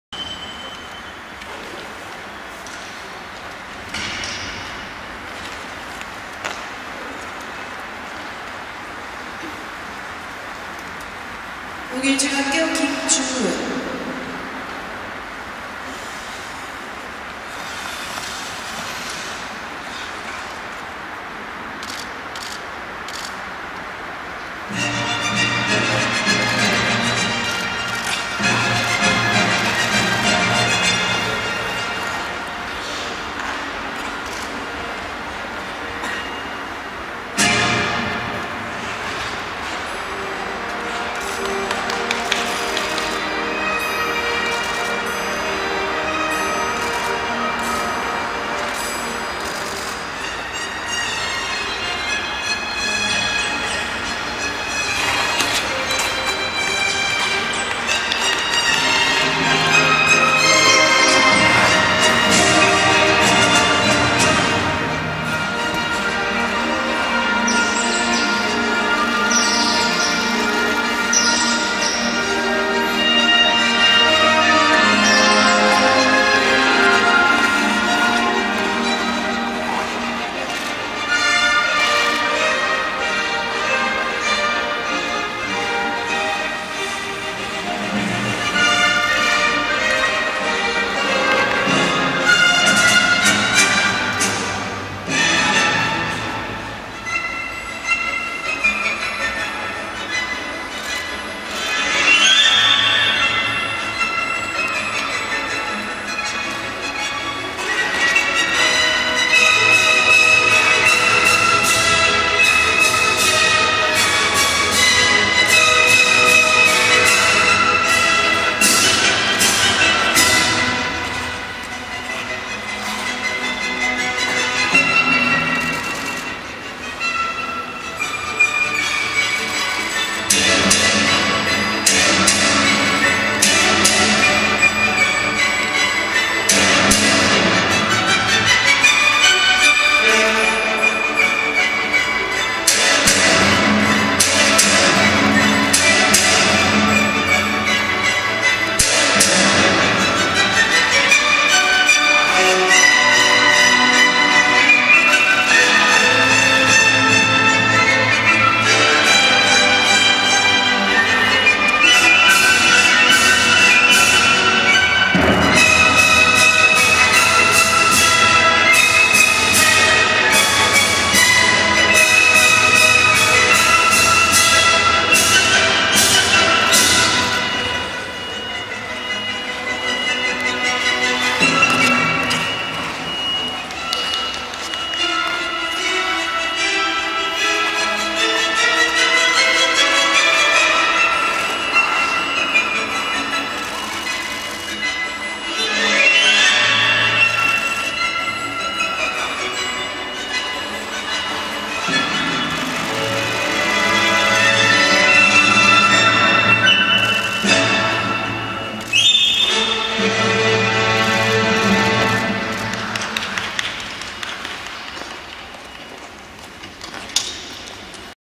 김주은 FS (목일중1, 3급) | 2012-11-11 서울시교육감배 | 여중부 C조 05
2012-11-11  제5회 서울특별시교육감배 겸 제30회 서울특별시빙상경기연맹회장배 피겨스케이팅대회 | 2일차 |
서울 목동아이스링크